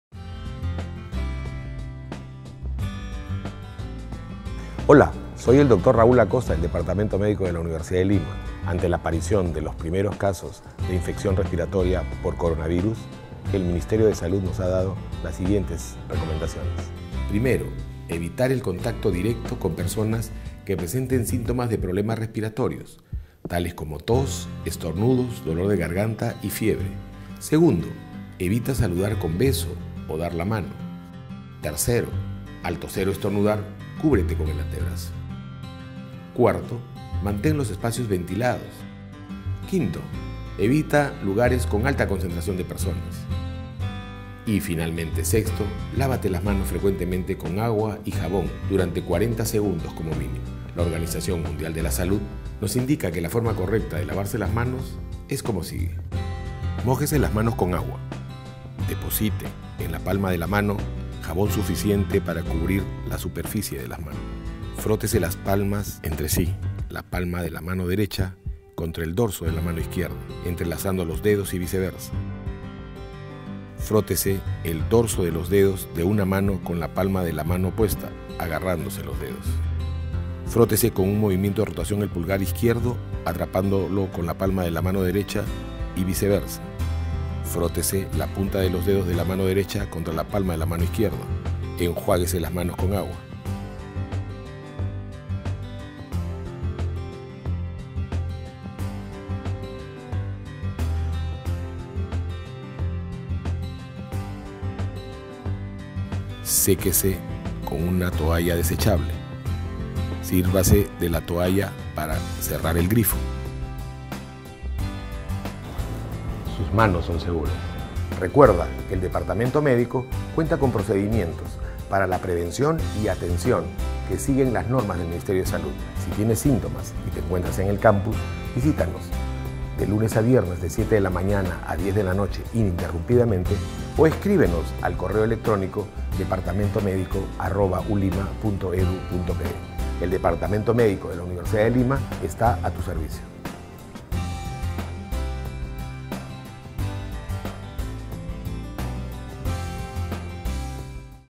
ULIMA - Departamento Médico Ulima
Nuestro Departamento Médico se encuentra a tu disposición. Además está siempre preparado para atender y cuidar a nuestra comunidad Ulima. En tal sentido, nos comparte recomendaciones para mantener una correcta higiene y prevenir casos de infección respiratoria por coronavirus (COVID-19).
Recuerda que al seguir estas pautas estaremos protegiéndonos a nosotros y a nuestros amigos y familiares.